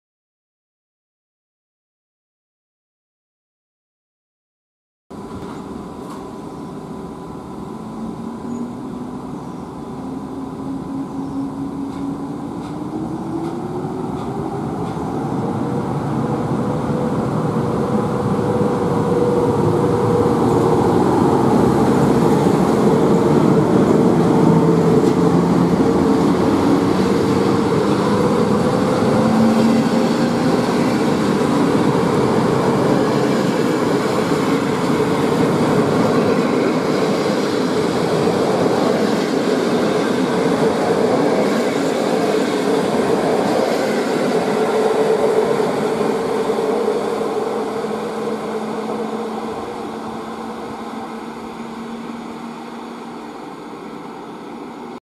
EP09-046 z IC Matejko | Łańcut 02.07.2022
OPIS
Siema!

Dziś film z Łańcuta.
Odjazd EP09-046 z IC Matejko z Łańcuta skromnie ale zawsze coś :)
SKŁADY
0:00 zdjęcie
0:05 EP09-046 z IC 3804 "MATEJKO" rel:Przemyśl Gł.-Szczecin Gł.
nagrane za pomocą:
Panasonic HC-V180